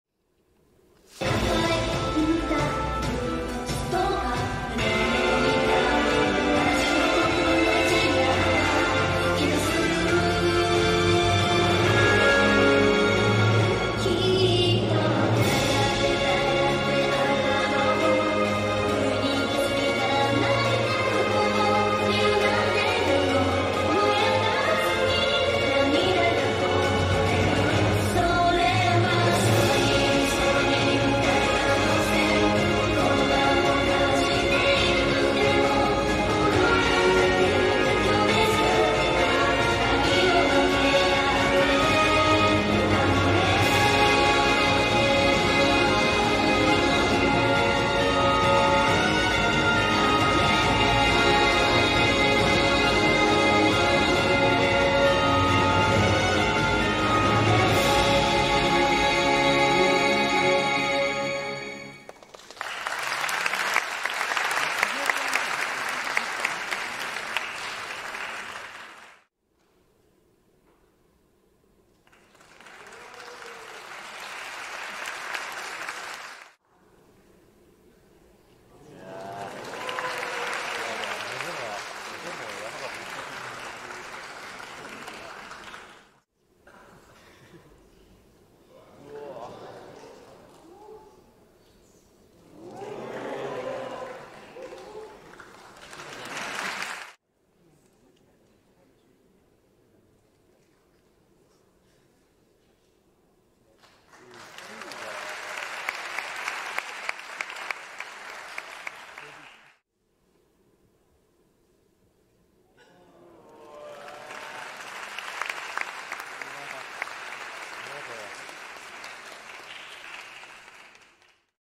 2022年 関東学生ボディビル選手権 優勝 刈川啓志郎 フリーポーズ
2022年 関東学生ボディビル選手権 優勝 刈川啓志郎選手のフリーポーズの映像です。